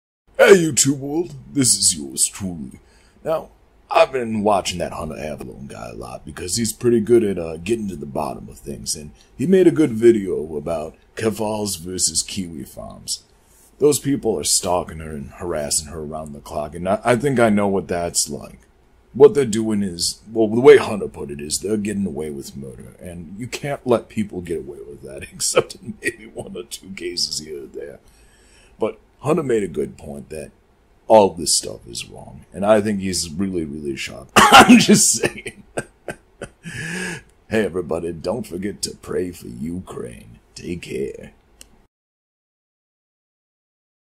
OJ Simpson (Parody) on Kiwi Farms and Keffals
OJ Simpson has an opinion on everything, and he has something to say on Keffals and Kiwi Farms!